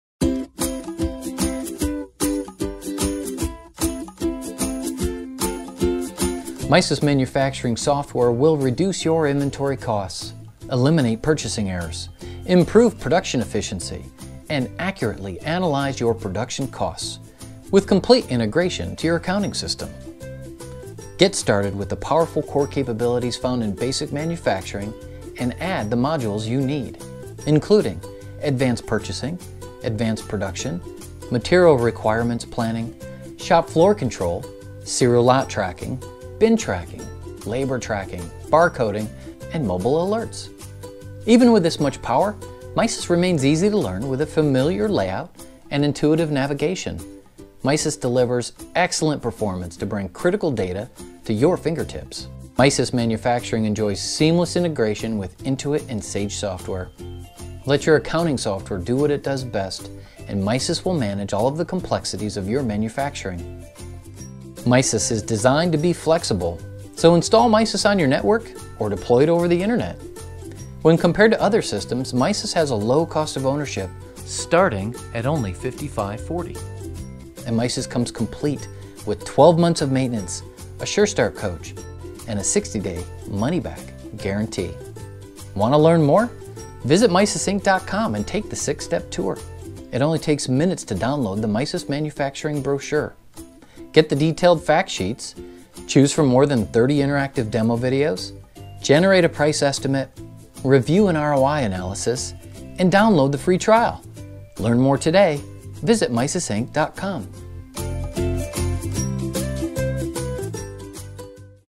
Intro to MISys Manufacturing MRP Software
MISys Manufacturing software offers all the functionality a small to medium-sized manufacturing firm needs today.

Get started with Basic Manufacturing and experience how MISys Manufacturing software delivers on its promise to turn your accounting software into an Industrial-Strength Manufacturing System. The Basic Manufacturing software provides the following capabilities:

- Extensive tracking of manufacturing inventory and work in process (WIP)
- Multi-level bills of material with full explosion down to 16 levels deep.
- Net-change physical inventory with cycle counting.
- Batch-oriented or direct entry of all stock transfer transactions.
- Full back-flushing of all assembly transactions.
- Choice of standard, average, LIFO, or FIFO costing methods.
- Cost adjustment with roll-up through all BOM levels.
- Work orders to document and track production activity.
- Extensive reporting using industry-standard report generator (Crystal Reports).
- Record detailed stock status for all inventoried items.
- Track inventory in multiple locations.
- Maintain inventory levels with minimum, maximum, and reorder points.
- Instantly display transaction history, where-used (BOM implosion), and outstanding order status.
- Can optionally allow or prevent negative inventory levels
- Associate a unique set of general ledger accounts with by specific item, job, or location